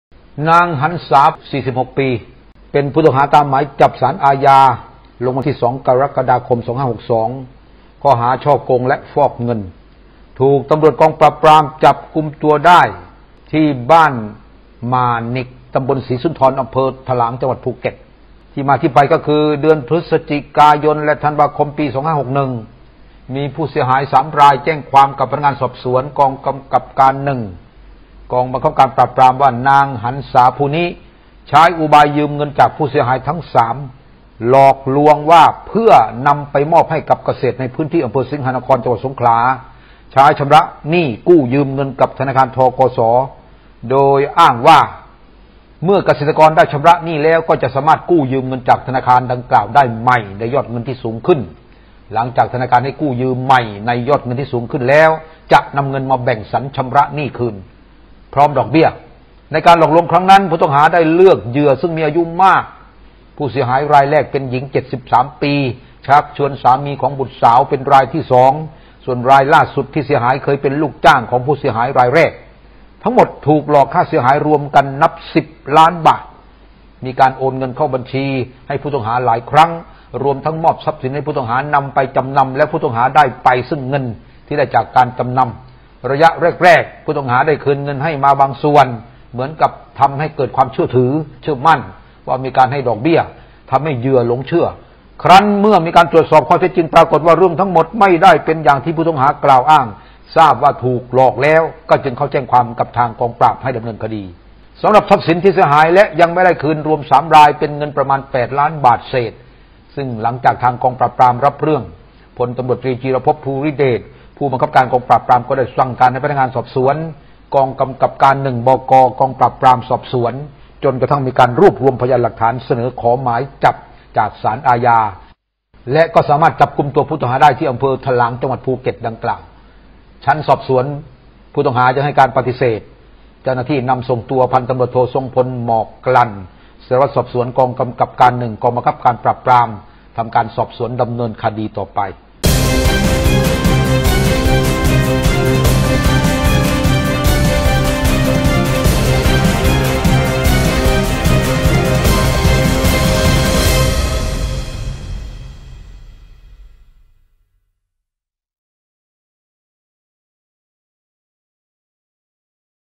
กองปราบจับสาวหลอกตุ๋นเหยื่ออ้างยืมเงินไปให้เกษตรจ่ายธนาคาร
กองปราบจับสาวหลอกตุ๋นเหยื่ออ้างยืมเงินไปให้เกษตรจ่ายธนาคาร สูญเงินไปร่วม 10 ล้านบาท